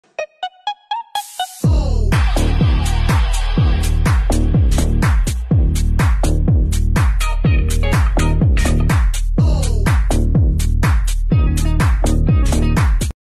Frameless black steel pivot interior glass door
Here you can see the operation of our frameless black steel pivot interior glass door from one of the UK's foremost interior glass door suppliers.